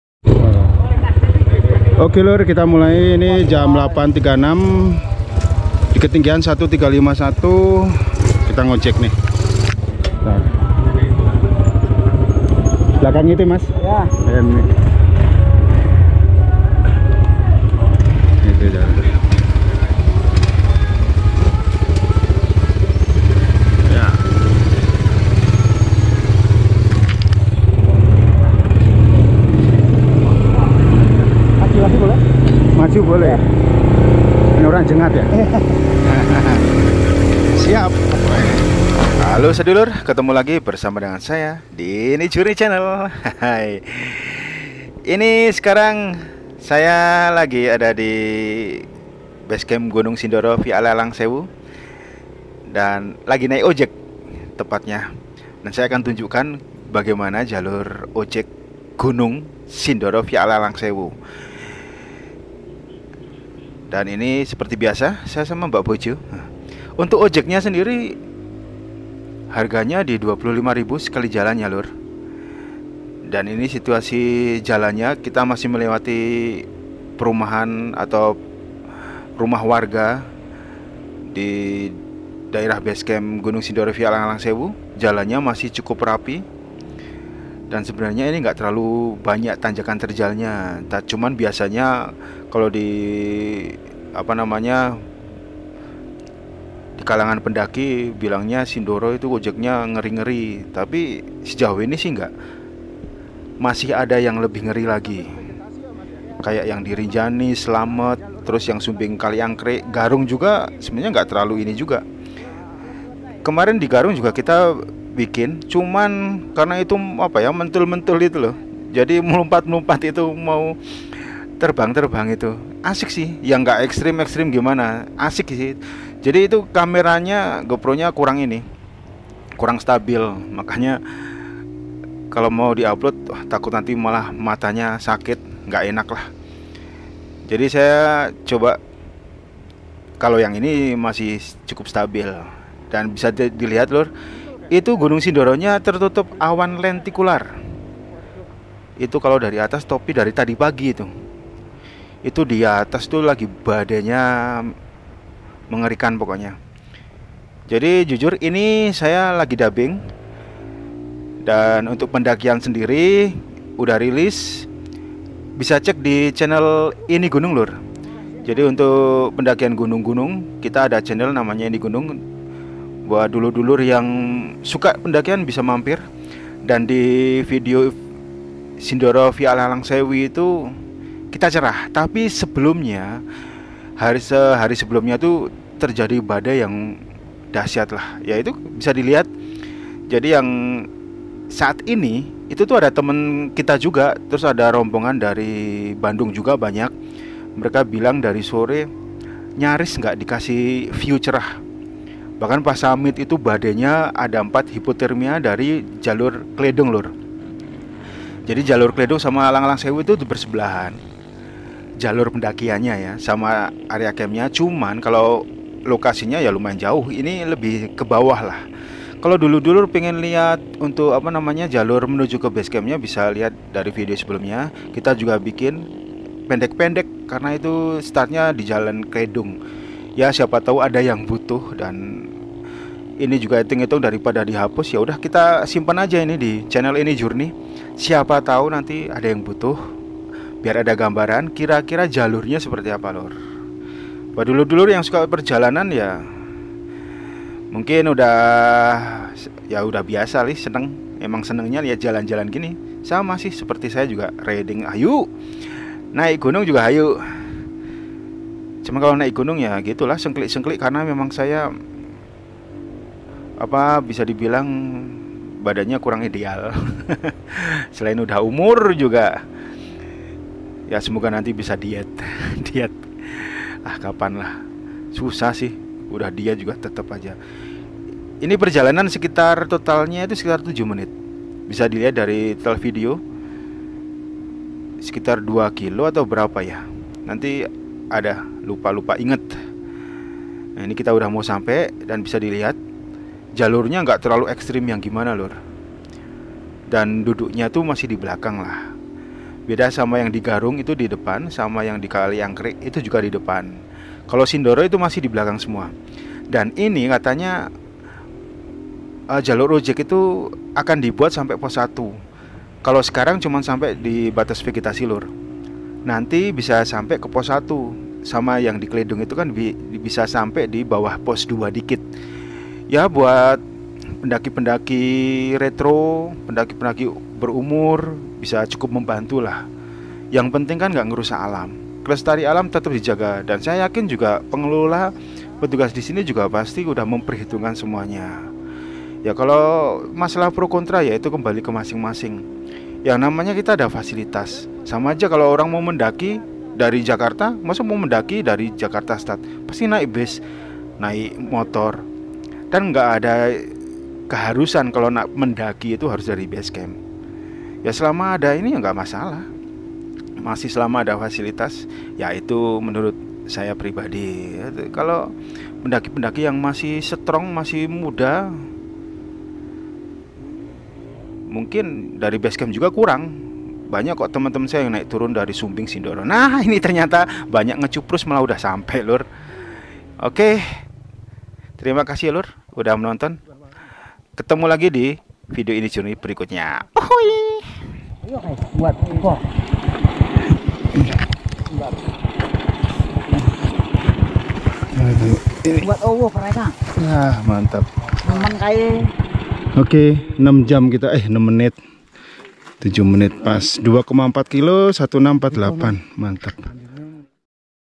OJEK GUNUNG SINDORO VIA ALANG - ALANG SEWU | Full Jalur Dari Basecamp Sampai Batas Vegetasi
TARIF OJEK GUNUNG SINDORO VIA ALANG - ALANG SEWU : 25 RIBU / ORANG

Basecamp Sindoro Via Alang Alang Sewu
Anggrunggondok, Jl. Sindoro Sumbing No.KM 07, RT.01/RW.03, Reco, Kec. Kertek, Kabupaten Wonosobo, Jawa Tengah 56371

GUNUNG SINDORO VIA ALANG - ALANG SEWU | Nekat Mendaki Saat Badai Lenticular & Summit Cuman Berdua